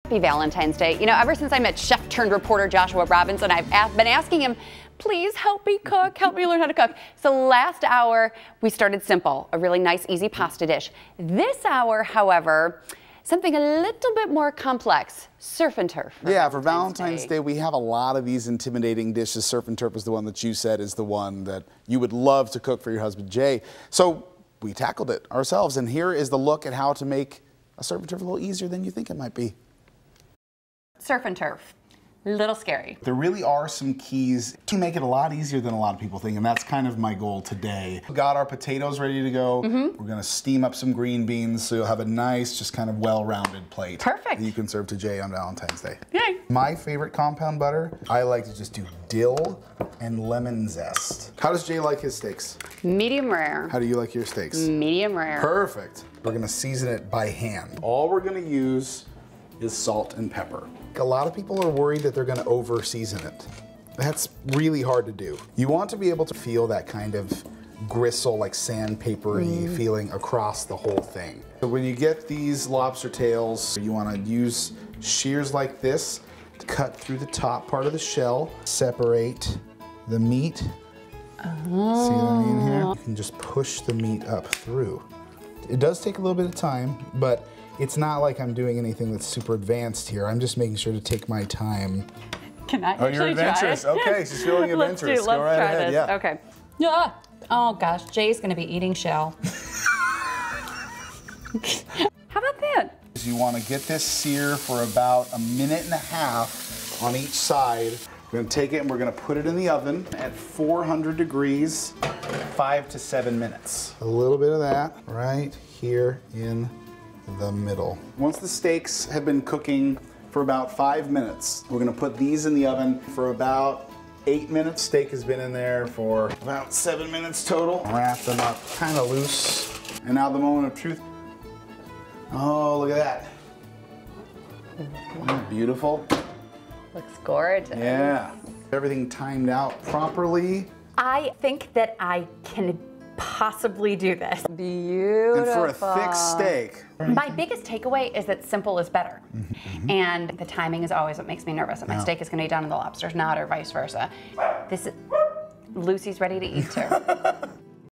Daybreak Does Valentine's Day Dinner: Surf and Turf
Daybreak's Joshua Robinson shows Melissa how to tackle surf and turf for Valentine's Day dinner, which might be easier than you think.